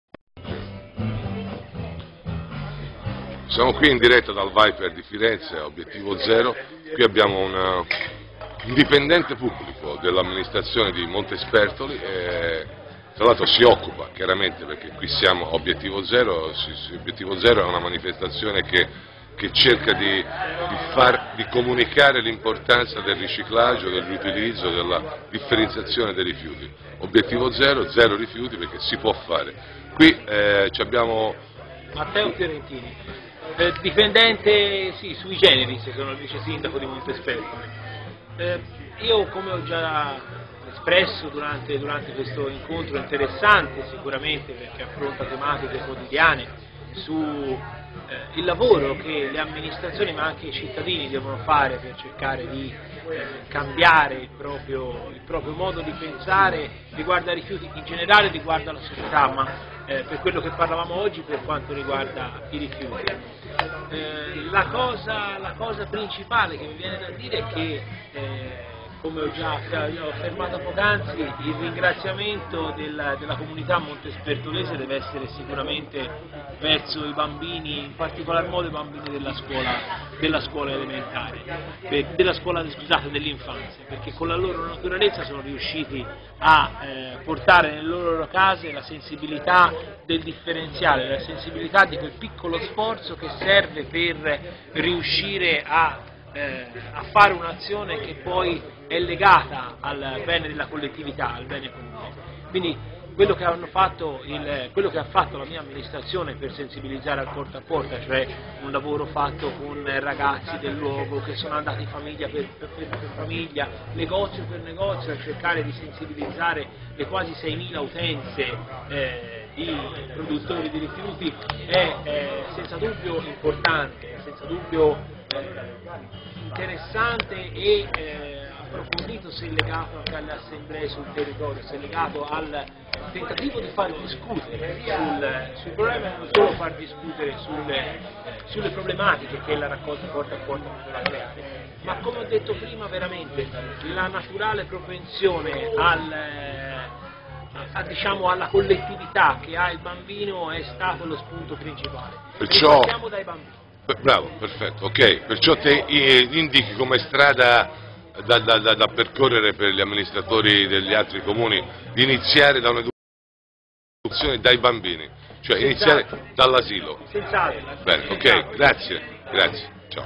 Montespertoli Educazione Ambientale
Le eccellenze toscane nel Porta a porta"
Le amministrazioni più virtuose in ambito di gestione dei rifiuti si incontrano pubblicamente per raccontare le loro esperienze, i punti di convergenza e divergenza, e soprattutto gli obiettivi per il futuro.
Intervengono: Alessio Ciacci (Comune di Capannori), Giuseppe Tartarini (Comune di Seravezza), Matteo Fiorentini (Comune di Montespertoli), Maria Elisabetta Sabatini (Comune di Follonica)
Il vicesindaco di Montespertoli forte dell'esperienza nel suo Comune con seimila utenti, di educazione e ambientale individuale ai nuclei familiari.
Alfatezzazione, ambientale e suggerisco culturale, tecnologica e spirituale universale.
Buone Pratiche, percorsi esperienziali, documenti, video e incontri con esperti, concerti e tanta gente!

Streamed by valerio polidori in Italy.